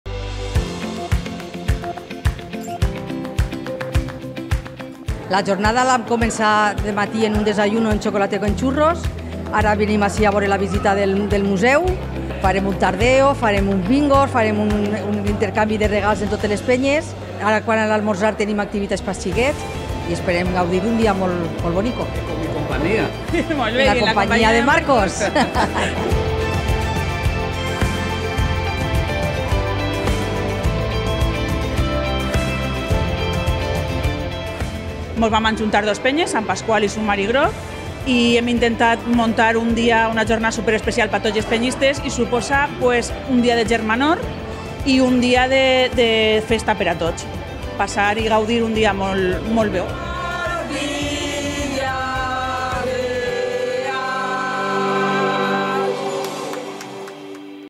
III Concentración de Peñas APV Villarreal CF
La Agrupació de Penyes del Villarreal CF ha celebrado la tercera edición de la Concentración de Peñas del Villarreal CF, impulsada esta temporada por las peñas Submarí Groc y Sant Pasqual, ambas de Vila-real.